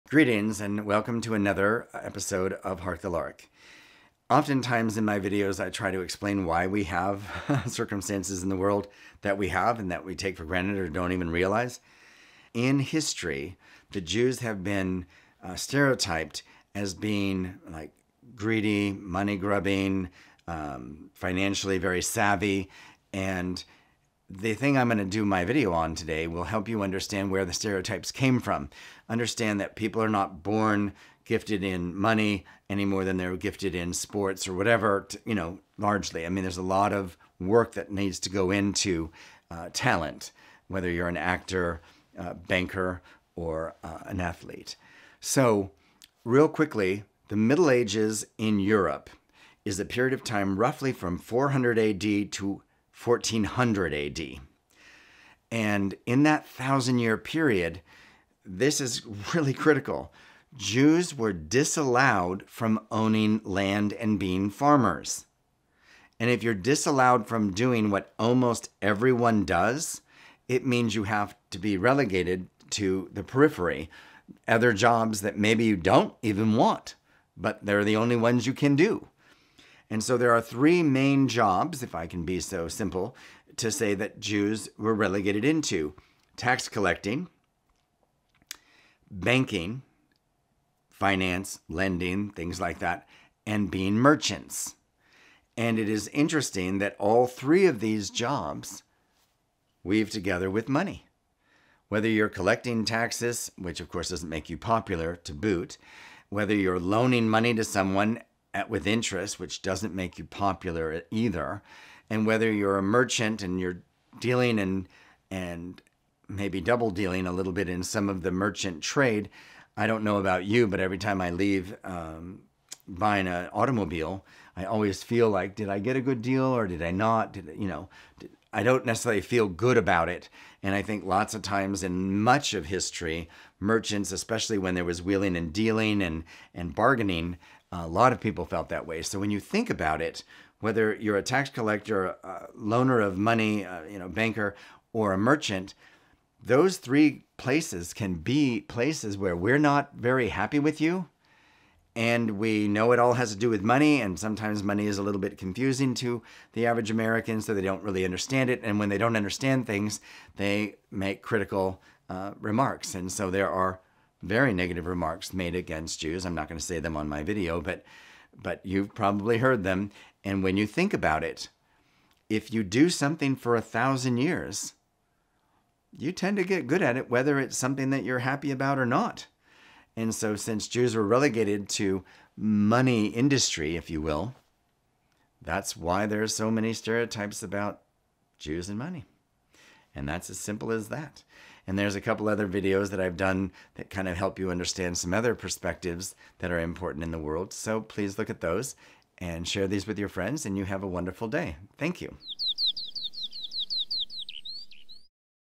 Jewish Stereotypes with money and from where they originated
In today's episode we discuss the origins of world wide spread Jewish stereotypes.